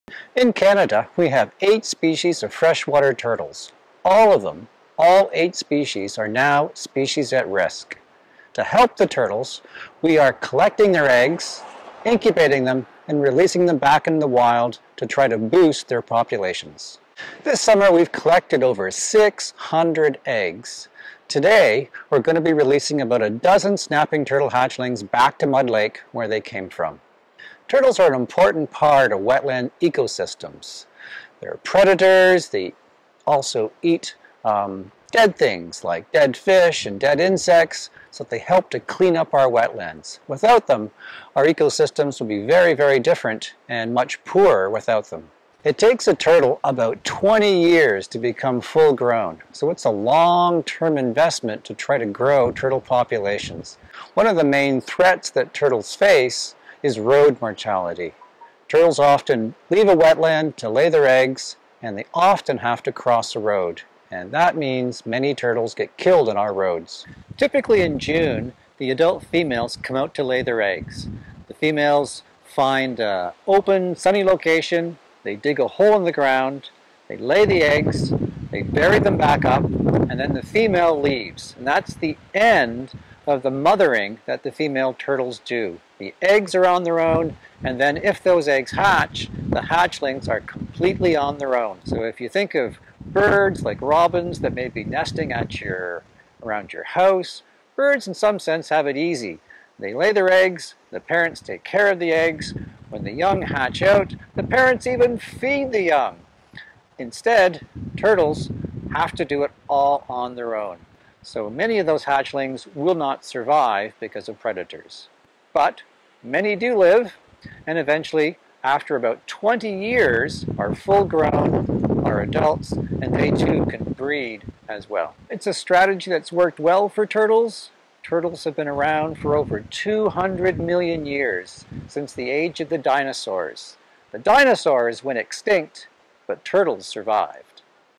Call of the wild: Turtle hatchlings released at Mud Lake
Thanks to the support of Ottawa-area residents, more than 600 turtle eggs were collected by the Canadian Wildlife Federation and incubated in a Kanata facility this year. David Seburn, a freshwater turtle specialist with the CWF, released a dozen hatchlings into Mud Lake, in Ottawa, Wednesday to help bolster the endangered population.